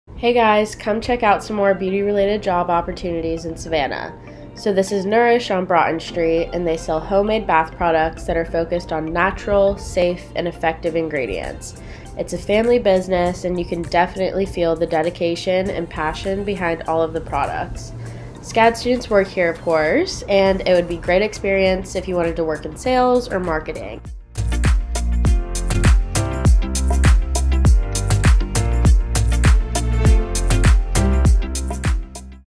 Explore Local Beauty-related Job Options as a SCADBeauty Student| Nourish
Come with me to check out Nourish! They are a natural bath product store here in Savannah, Georgia!